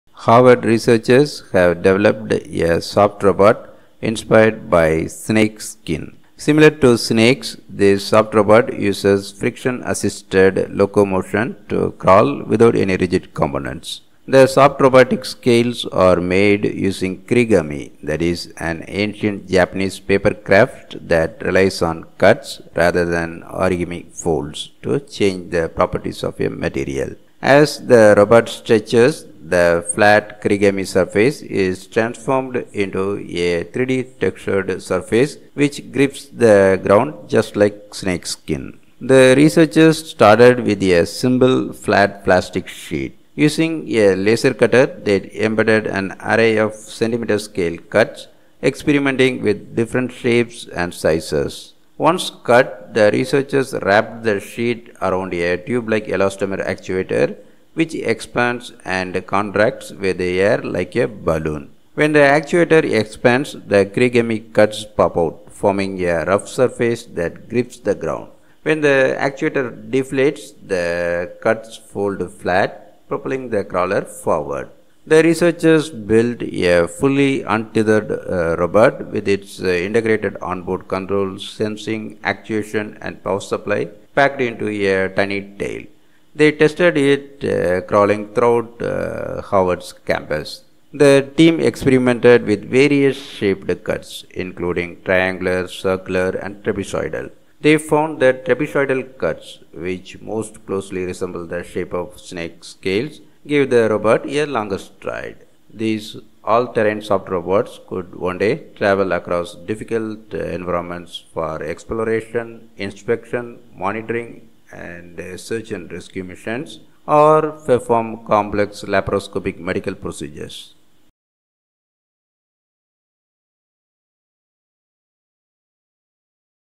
Harvard's Snake-inspired Robot uses Kirigami to move | QPT
Harvard researchers have developed  a  soft robot inspired by snakeskin.  Similar to Snakes,  this Soft Robot uses  friction-assisted locomotion to crawl without any rigid components.

The  soft robotic scales are made using kirigami - an ancient Japanese paper craft that relies on cuts, rather than origami folds, to change the properties of a material. As the robot stretches, the flat kirigami surface is transformed into a 3D-textured surface, which grips the ground just like snakeskin.

The researchers started with a simple, flat plastic sheet. Using a laser cutter, they embedded an array of centimeter-scale cuts, experimenting with different shapes and sizes. Once cut, the researchers wrapped the sheet around a tube-like elastomer actuator, which expands and contracts with air like a balloon.
When the actuator expands, the kirigami cuts pop-out, forming a rough surface that grips the ground. When the actuator deflates, the cuts fold flat, propelling the crawler forward.

The researchers built a fully untethered robot, with its integrated on-board control, sensing, actuation and power supply packed into a tiny tail. They tested it crawling throughout Harvard’s campus.

The team experimented with various-shaped cuts, including triangular, circular and trapezoidal. They found that trapezoidal cuts — which most closely resemble the shape of snake scales —gave the robot a longer stride.

These all-terrain soft robots could one day travel across difficult environments for exploration, inspection, monitoring and search and rescue missions or perform complex, laparoscopic medical procedures

Video courtesy of Ahmad Rafsanjani/Harvard SEAS